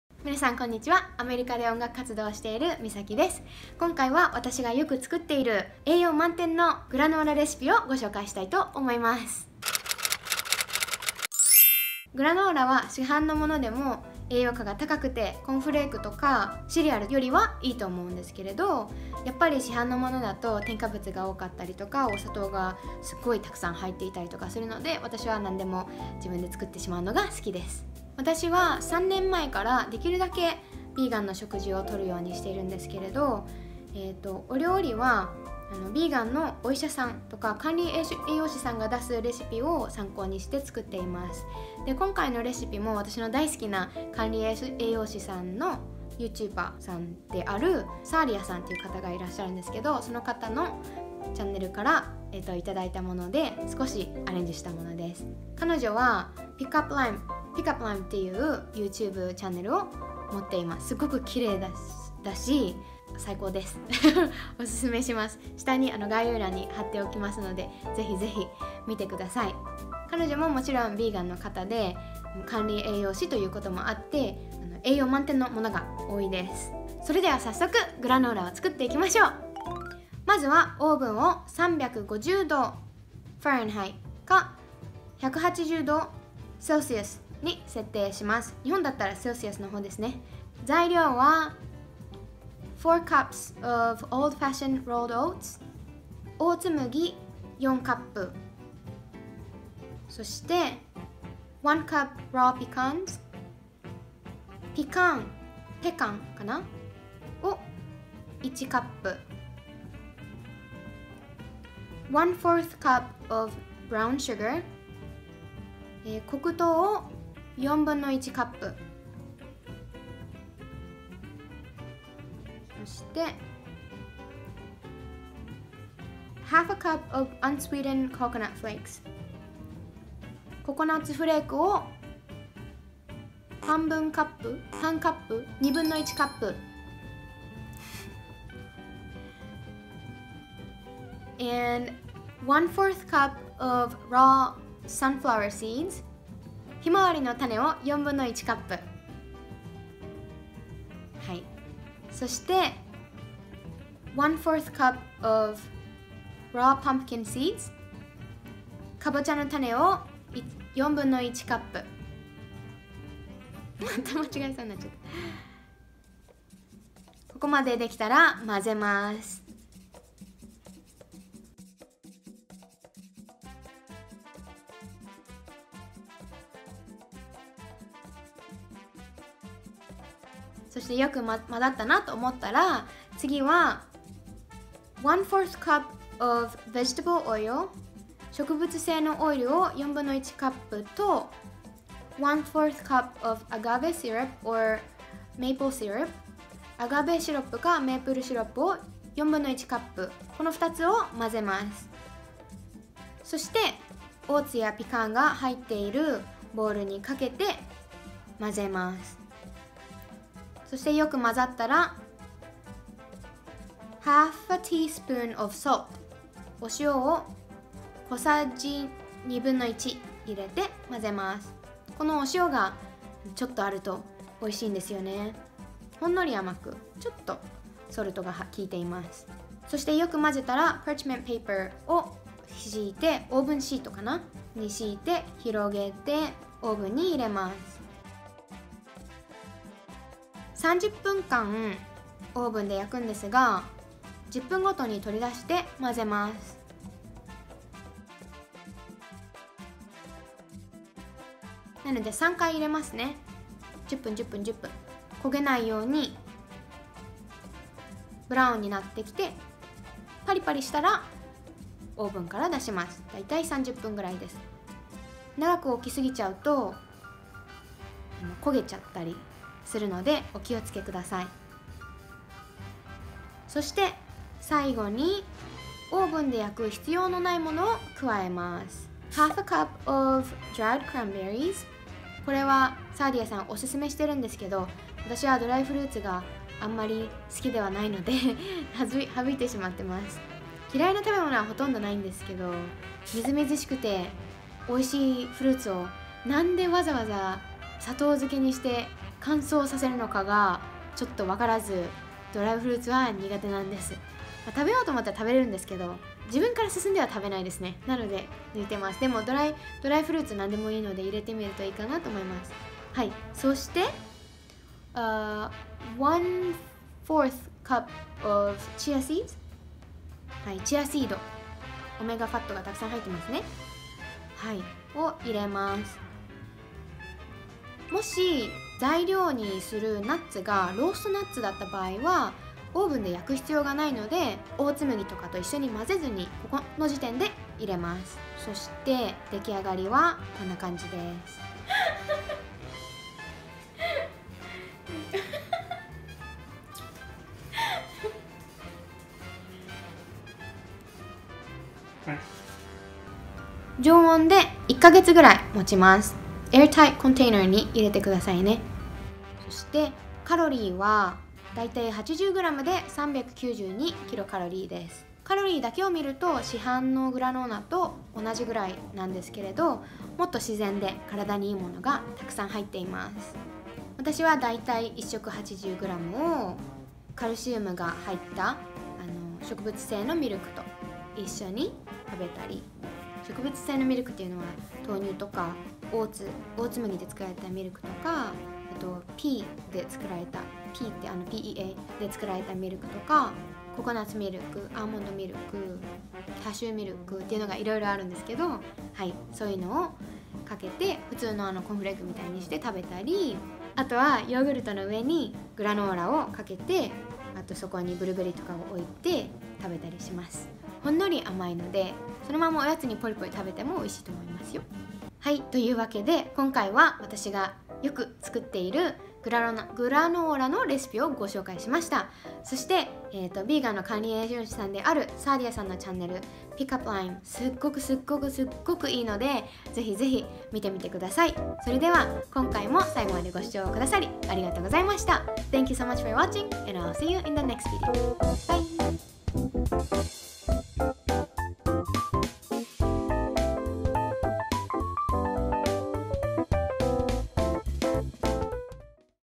グラノーラの作り方！オートミールで作るアレンジレシピ🎶簡単ヘルシー＆ビーガン！朝ごはんにも！(#68)
今回は初めてレシピ動画を撮ってみました！普段よく作っているヘルシーなグラノーラのレシピをご紹介します！次回はもっと上手に撮れるように工夫します〜

Table of contents/目次
0:00 introduction
0:11 手作りする理由、ビーガンの管理栄養士さんのご紹介
1:19 作り方
5:42 できた！完成形

SNS/リンク